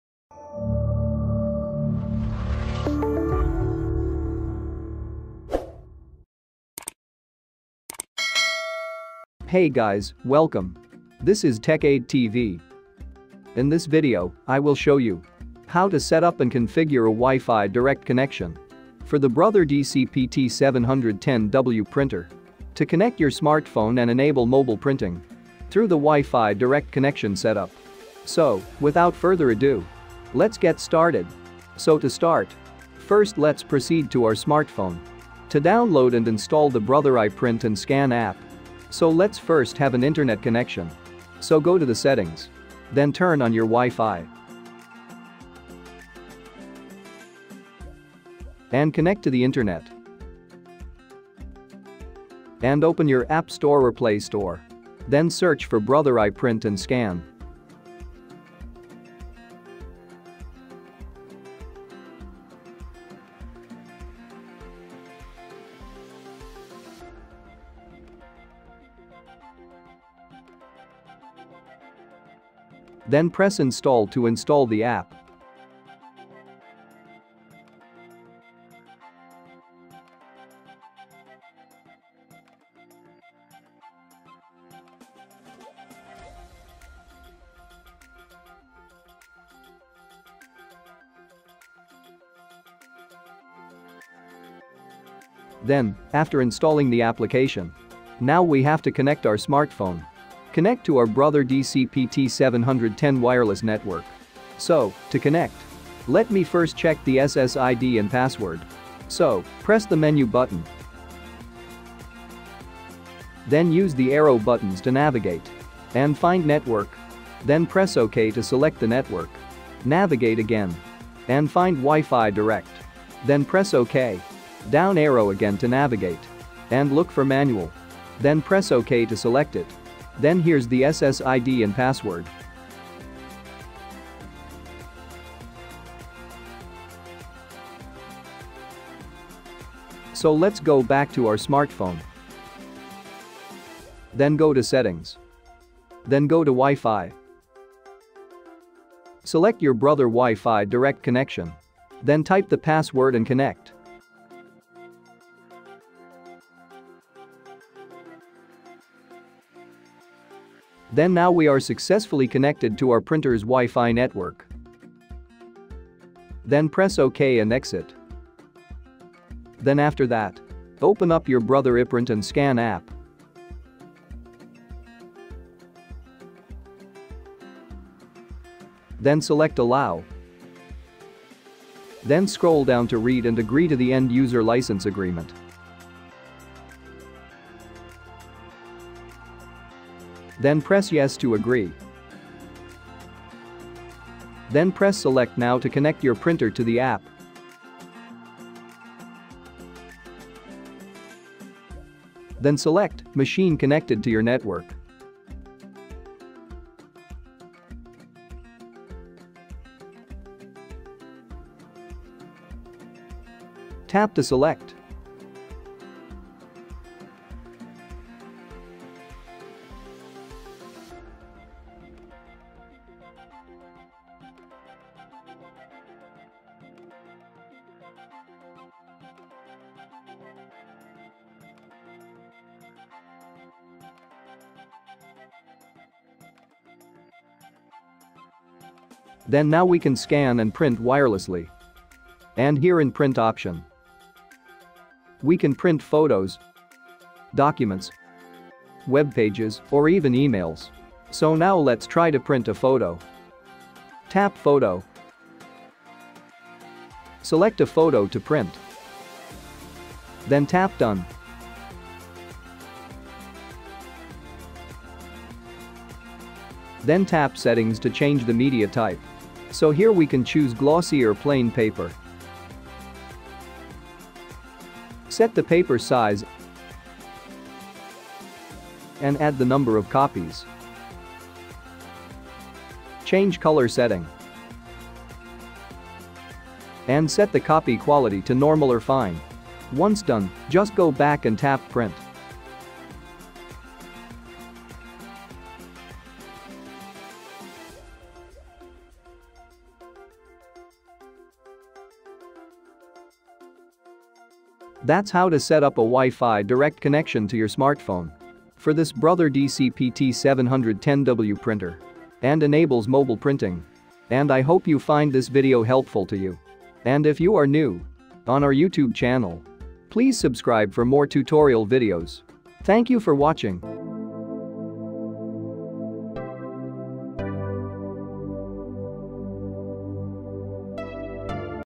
HOW TO SETUP WI-FI DIRECT AND PRINT USING SMARTPHONE - BROTHER DCP-T710W PRINTER.
This is the detailed and simple step-by-step procedure to setup and configure your Brother printer's Wi-Fi Direct connection to print wirelessly using your smartphone. Print from smartphone to Brother printer.

BrotherDCP-T710W
Wi-fi Configuration